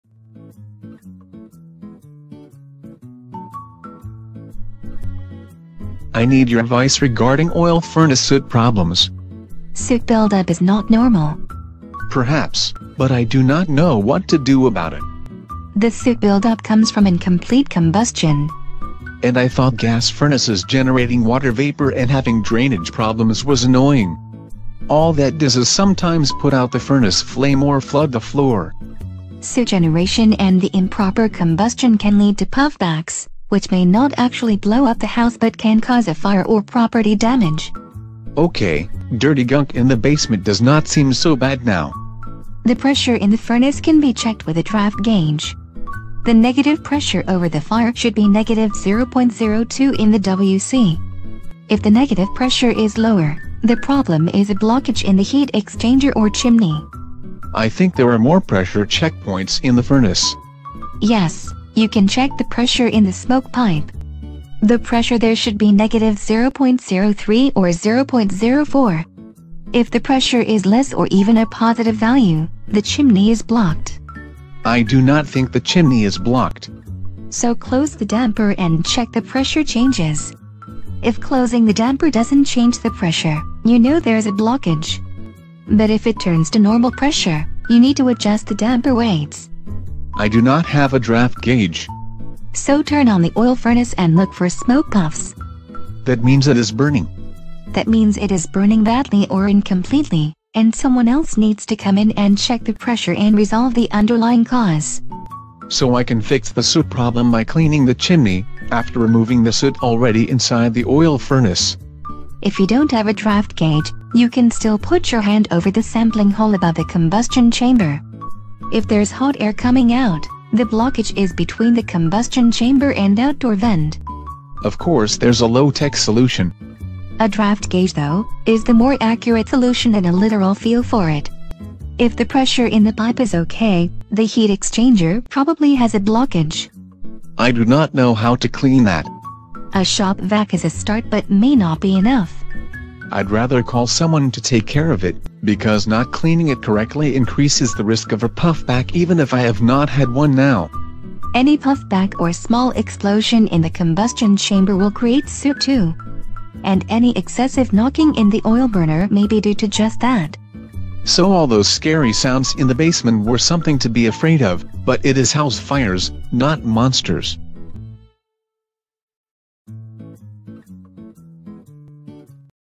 Oil Furnace Soot Problems
I need your advice regarding oil furnace soot problems.

Soot build up is not normal.

Perhaps, but I do not know what to do about it.

The soot build up comes from incomplete combustion.

And I thought gas furnaces generating water vapor and having drainage problems was annoying. All that does is sometimes put out the furnace flame or flood the floor.

Soot generation and the improper combustion can lead to puff backs, which may not actually blow up the house but can cause a fire or property damage.

Okay, dirty gunk in the basement does not seem so bad now.

The pressure in the furnace can be checked with a draft gauge; the negative pressure over the fire should be negative 0.02 in the wc. If the negative pressure is lower, the problem is a blockage in the heat exchanger or chimney.

I think there are more pressure check points in the furnace.

Yes, you can check the pressure in the smoke pipe. The pressure there should be negative 0.03 or 0.04. If the pressure is less or even a positive value, the chimney is blocked.

I do not think the chimney is blocked.

So close the damper and check the pressure changes. If closing the damper doesn’t change the pressure, you know there’s a blockage, but if it turns to normal pressure, you need to adjust the damper weights.

I do not have a draft gauge.

So turn on the oil furnace and look for smoke puffs.

That means it is burning.

That means it is burning badly or incompletely, and someone else needs to come in and check the pressure and resolve the underlying cause.

So I can fix the soot problem by cleaning the chimney, after removing the soot already inside the oil furnace.

If you don’t have a draft gauge, you can still put your hand over the sampling hole above the combustion chamber; if there’s hot air coming out, the blockage is between the combustion chamber and outdoor vent.

Of course there’s a low tech solution.

A draft gauge though, is the more accurate solution than a literal feel for it. If the pressure in the pipe is okay, the heat exchanger probably has a blockage.

I do not know how to clean that.

A shop vac is a start but may not be enough.

I’d rather call someone to take care of it, because not cleaning it correctly increases the risk of a puff back even if I have not had one now.

Any puff back or small explosion in the combustion chamber will create soot too. And any excessive knocking in the oil burner may be due to just that.

So all those scary sounds in the basement were something to be afraid of, but it is house fires, not monsters.